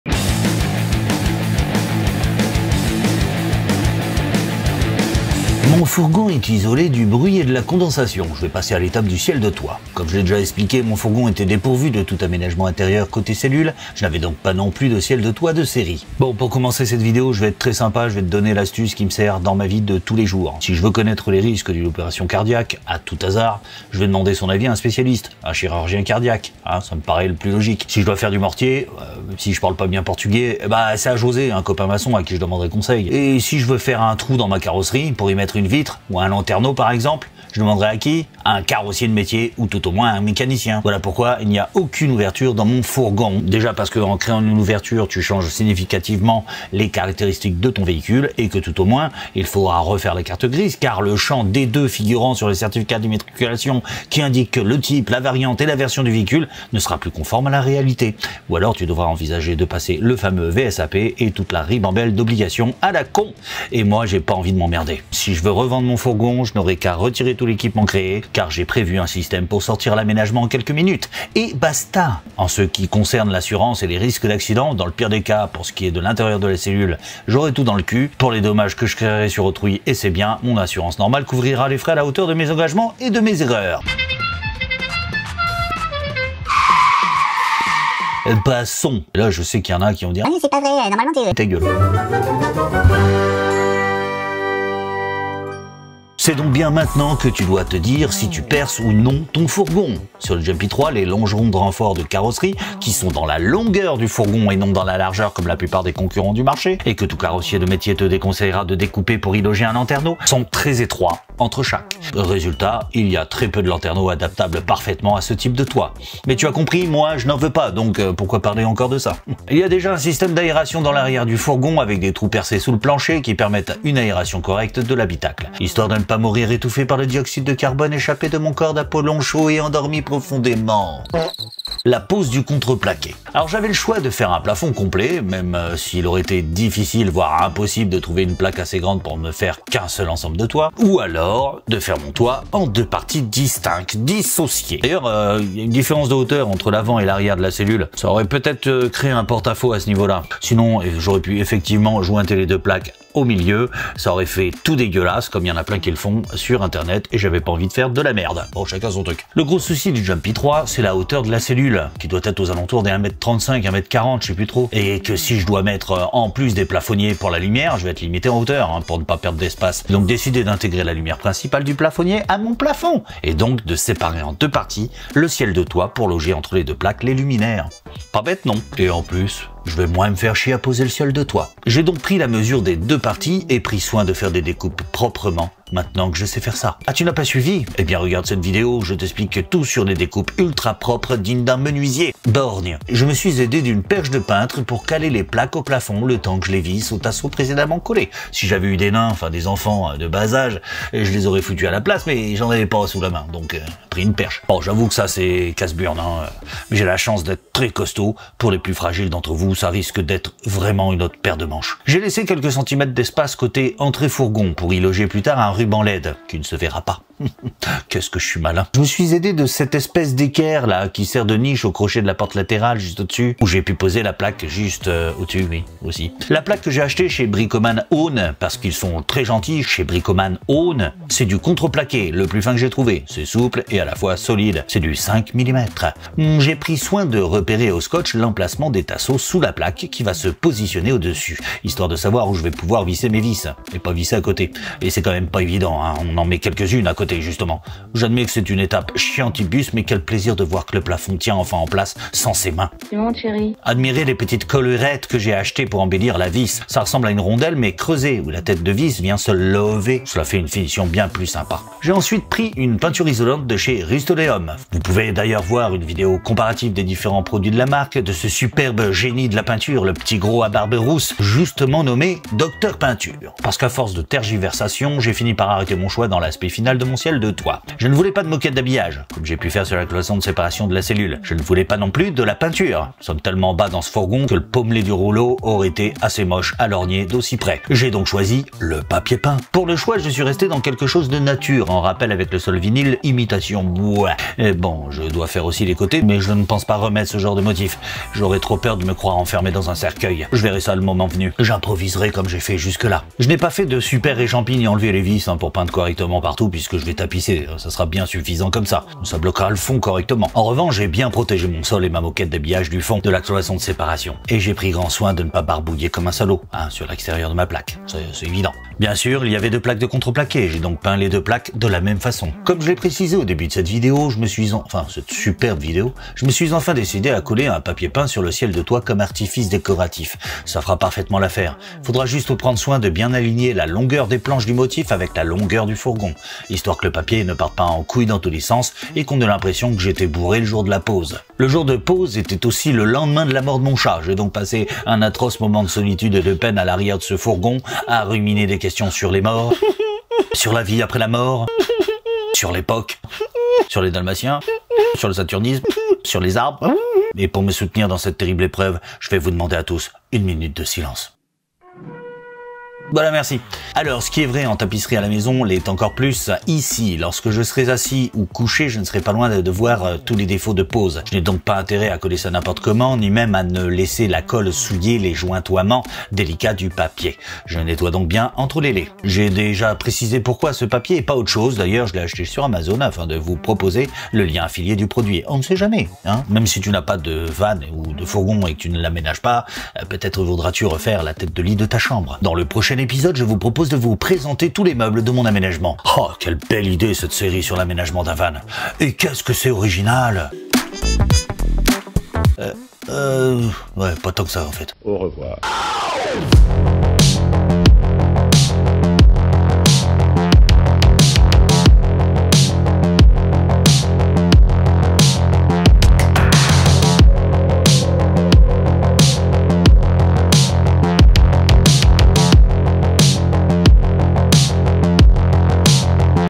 Le ciel de toit de mon fourgon | ça commence à ressembler à quelque chose
Après l'isolation, je vais enfin pouvoir plaquer mon ciel de toit ! ça n'est pas une mince affaire, surtout quand encore une fois, on n'a pas de gabarit ni de mesures précises...